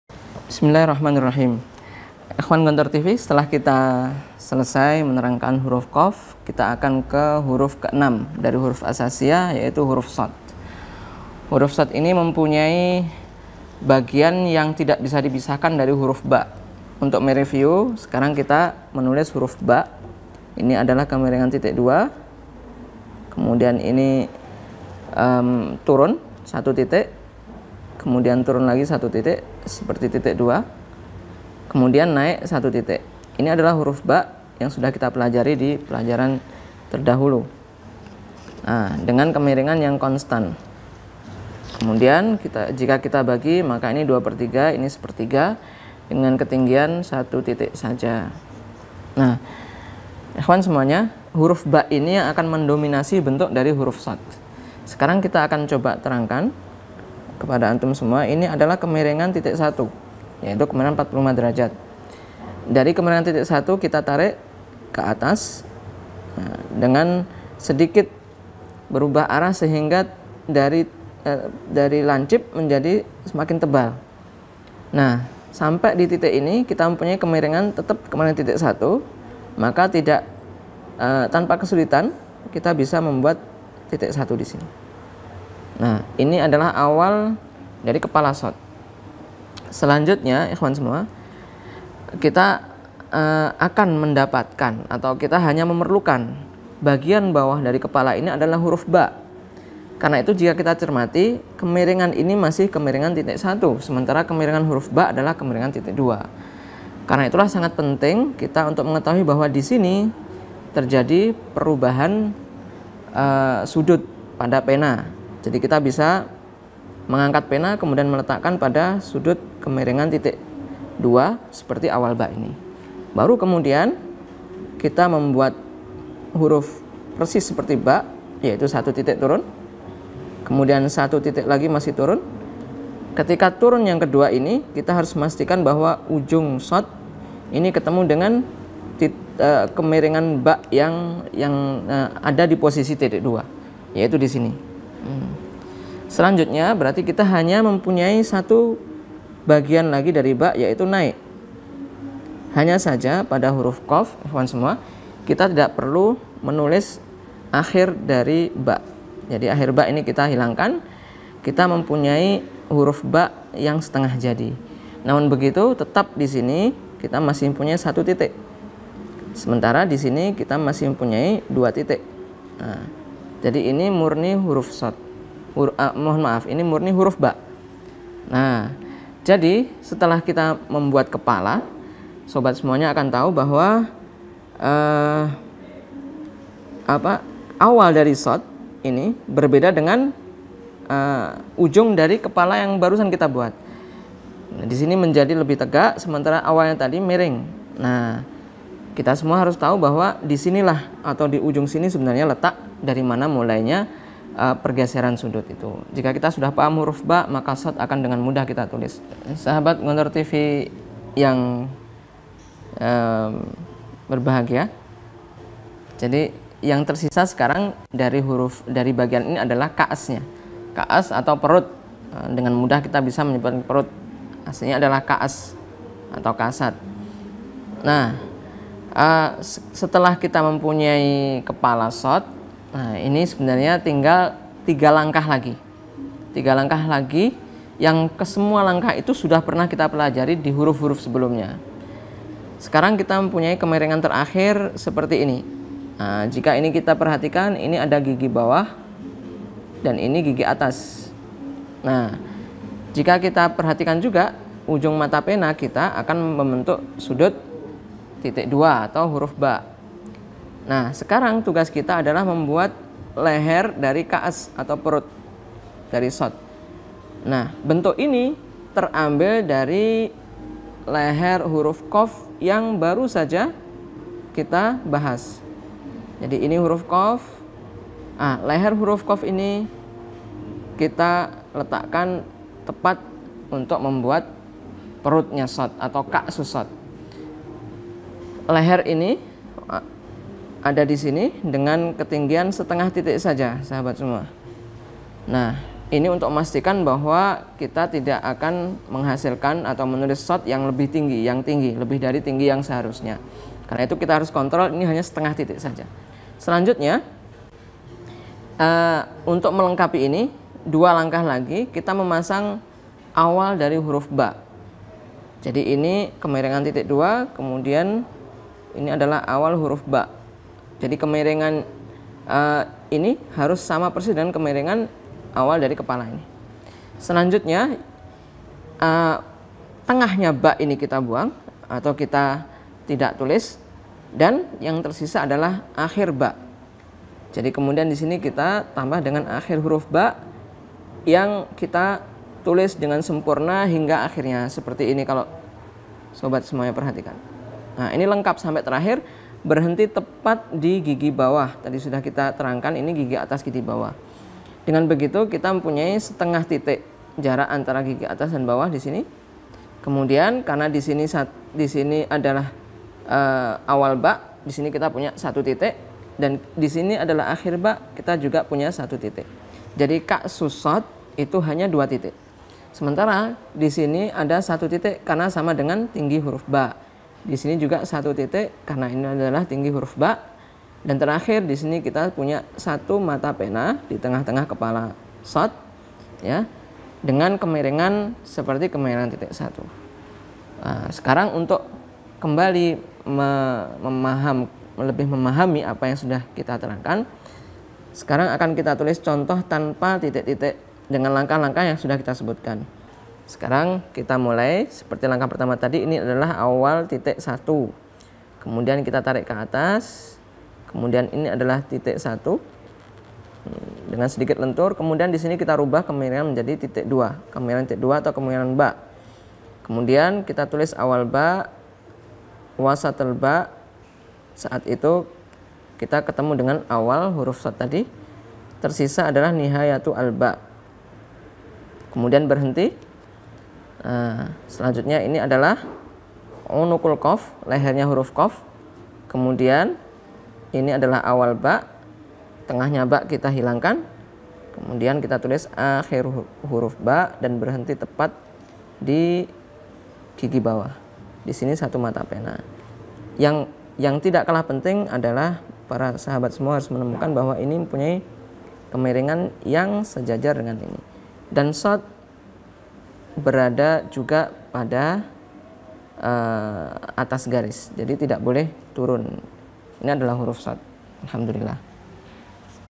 Cara Menulis Huruf Shad Khat Riq'ah - Ust Muhammad Nur Lc - Belajar Kaligrafi Eps 5 part 2
Also tagged with: Gontor TV, Pondok Pesantren, Edutainment, Islam, Muslim, Indonesia, Ponorogo, Jawa Timur, Pondok Modern, Pesantren,